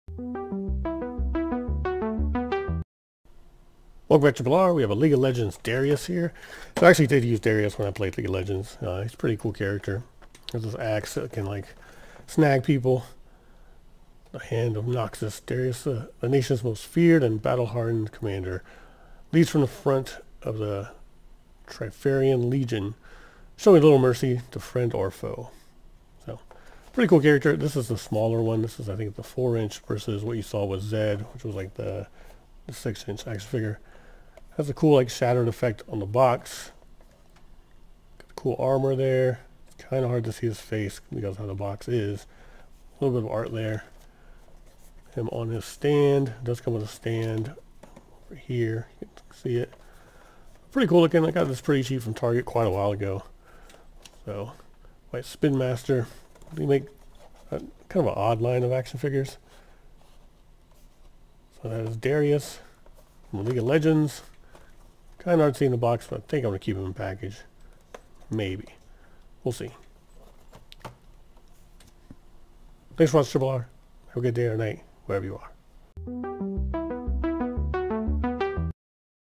League of Legends Darius
Calling all League of Legends enthusiasts! Feast your eyes on this amazing Darius action figure. Crafted with precision and care, this figure captures the essence of Darius, the Noxian Hand of Noxus. From his imposing stature to his fearsome weapon, every detail has been faithfully recreated. Whether you're a fan of Darius or simply appreciate finely crafted collectibles, this action figure is a must-have for your display shelf.